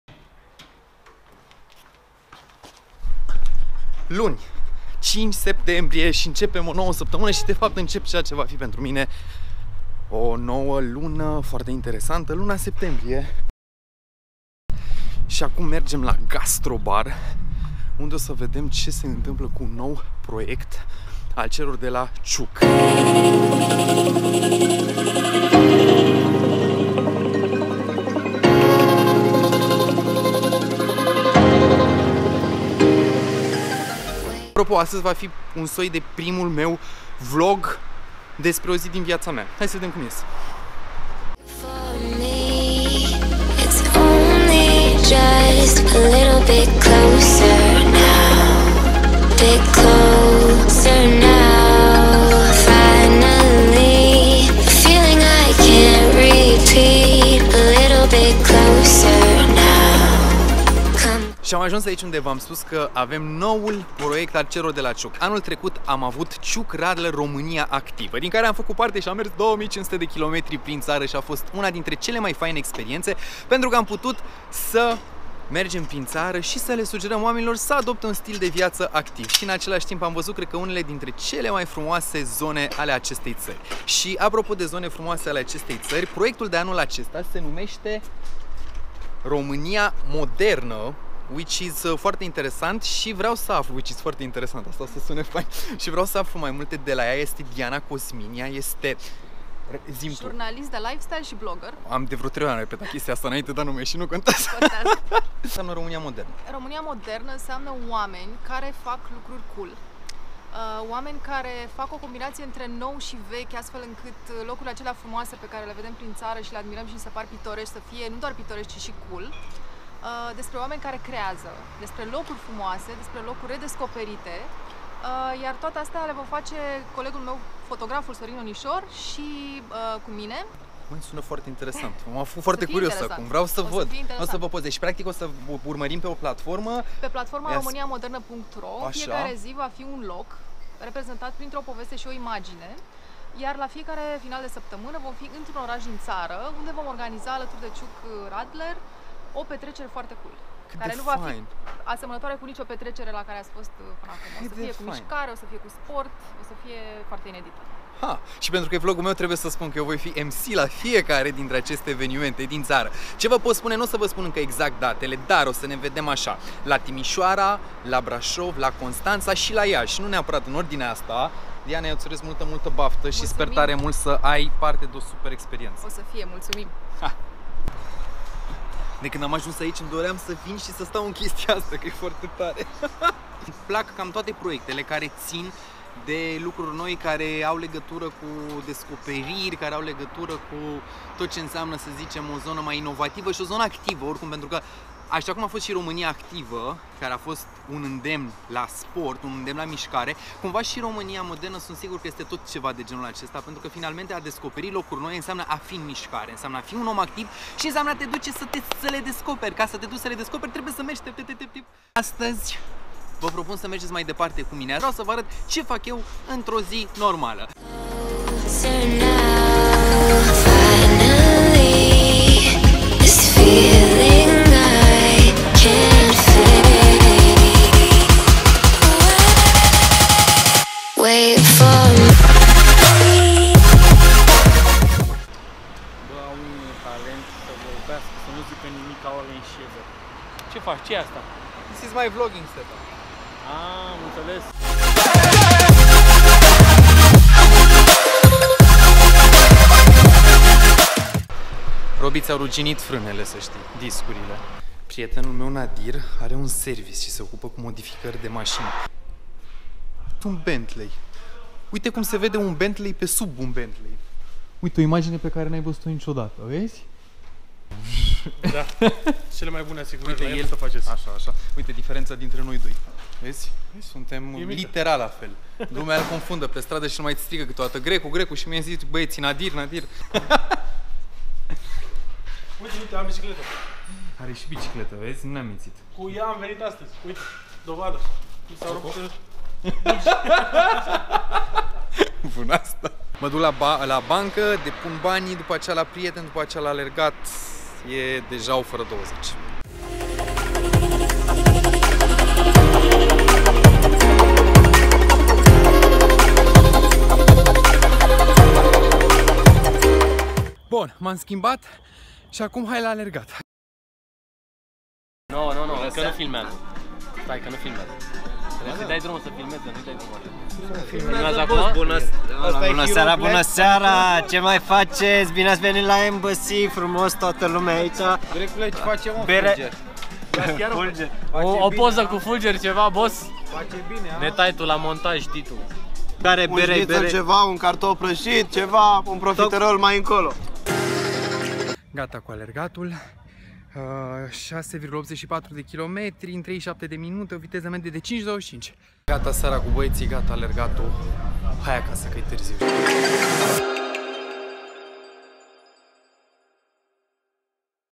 O zi modernă
Am aflat despre frumoasa inițitivă , m-am întâlnit cu oameni faini și am alergat! Vlog-ul unei zile moderne :)